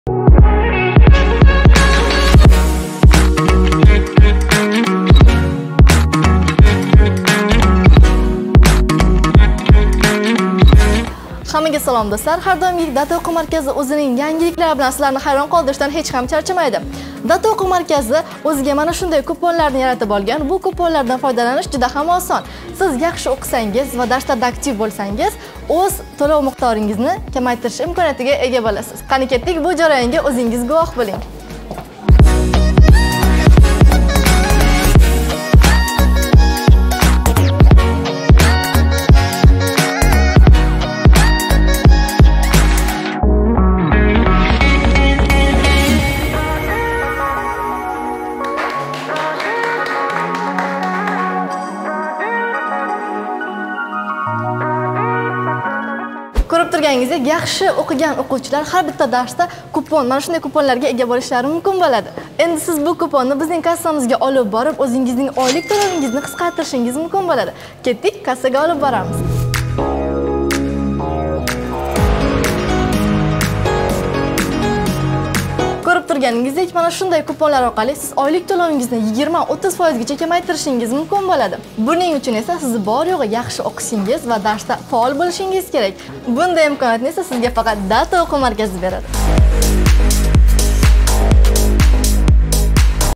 Faollik ko’rsatib chegirmalarga ega bo’ling
😉 «DATA» o’quv markazi va «Data kids» loyihasining darslari davomida doimiy tarzda 15, 20, 25 ming so’mlik chegirma sertifikatlari darsda aktiv qatnashgan va yaxshi o’zlashtirgan o’quvchilarga taqdim etilib kelinadi. Demak, o’zingizni namoyon etishingiz sizga endi yanada ko’proq foyda olib keladi.

💸 Bugungi video orqali ushbu chegirma sertifikatlariga ega bo’lish va ulardan foydalanish haqida ma’lumotga ega bo’lishingiz mumkin.